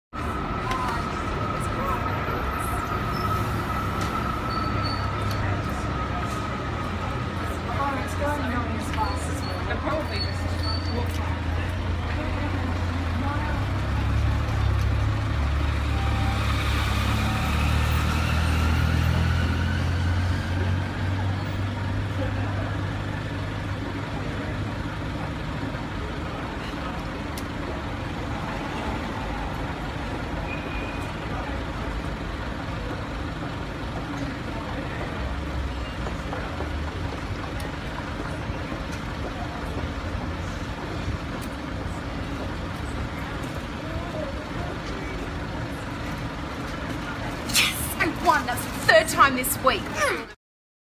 Pretending I won money at the ATM (for a bus)
Hello lovely people - this is a video for the Silly Things For A Bus Project raising money for Waratah SDS.

This video is dedicated to the lovely backers - Sam Leader, Amanda Kendle and Yishin Chan!